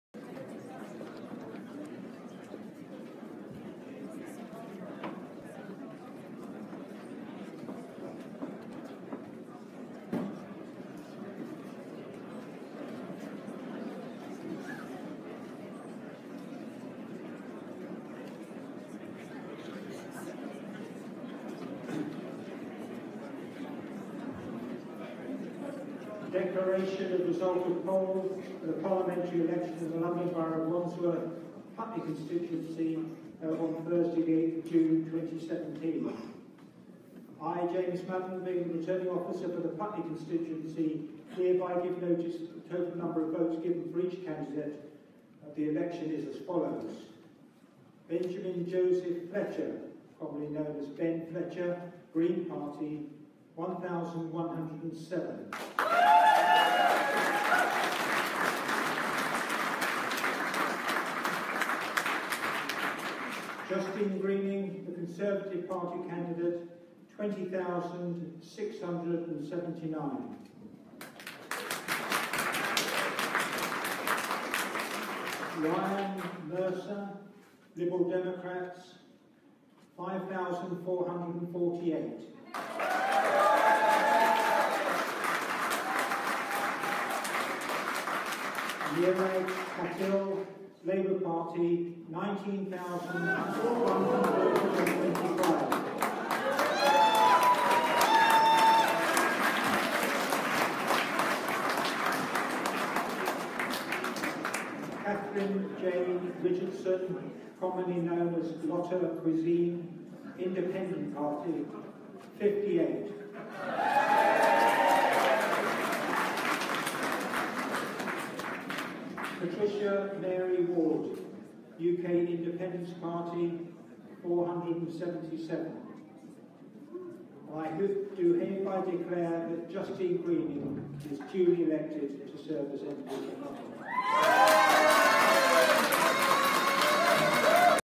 Putney constituency declaration General Election 2017
Live video of the announcement